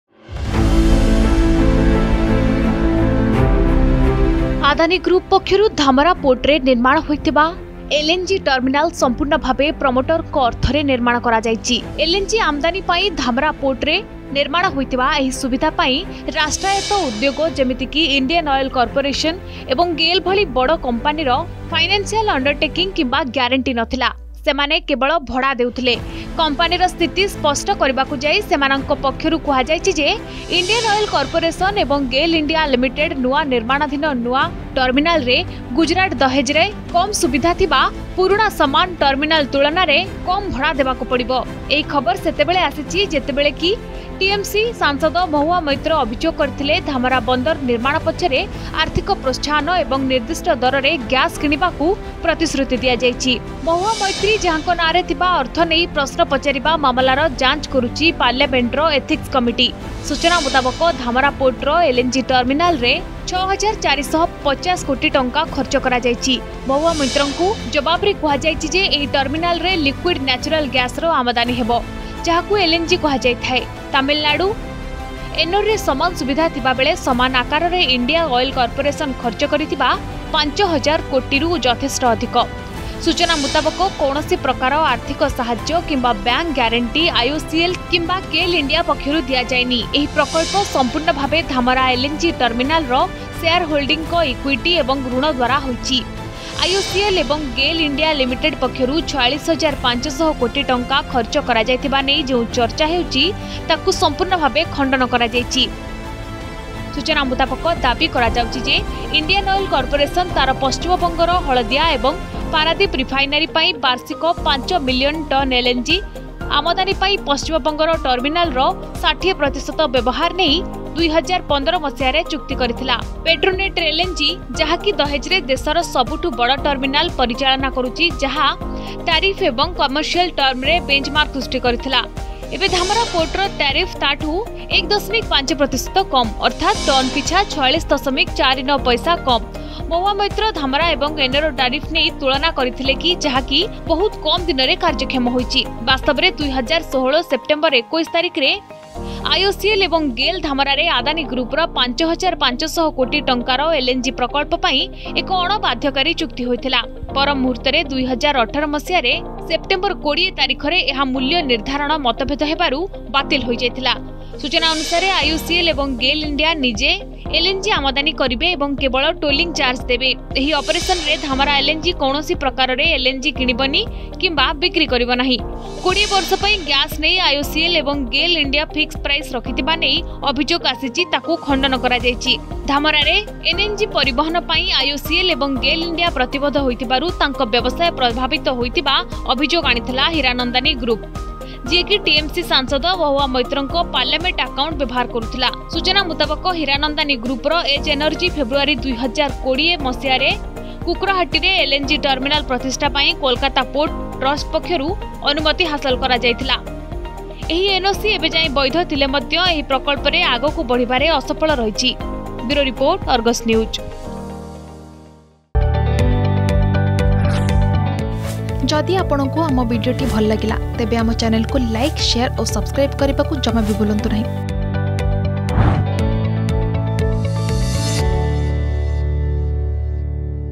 ଧାମରା ପୋର୍ଟରେ ଏଲଏନଜି ଟର୍ମିନାଲ୍ ସଂପୂର୍ଣ୍ଣ ଭାବେ ପ୍ରମୋଟରଙ୍କ ଅର୍ଥରେ ନିର୍ମାଣ || Adani Group
ଆଦାନୀ ଗ୍ରୁପ ପକ୍ଷରୁ ଧାମରା ପୋର୍ଟରେ ନିର୍ମାଣ ହୋଇଥିବା ଏଲଏନଜି ଟର୍ମିନାଲ୍ ସଂପୂର୍ଣ୍ଣ ଭାବେ ପ୍ରମୋଟରଙ୍କ ଅର୍ଥରେ ନିର୍ମାଣ କରାଯାଇଛି । ଏଲଏନଜି ଆମଦାନୀ ପାଇଁ ଧାମରା ପୋର୍ଟରେ ନିର୍ମାଣ ହୋଇଥିବା ଏହି ସୁବିଧା ପାଇଁ ରାଷ୍ଟ୍ରାୟତ୍ତ ଉଦ୍ୟୋଗ ଯେମିତିକି ଇଣ୍ଡିଆନ ଅଏଲ କର୍ପୋରେସନ ଏବଂ ଗେଲ୍ ଭଳି ବଡ କମ୍ପାନୀର ଫାଇନାନ୍ସସିଆଲ୍ ଅଣ୍ଡରଟେକିଂ କିମ୍ବା ଗ୍ୟାରେଣ୍ଟି ନଥିଲା ।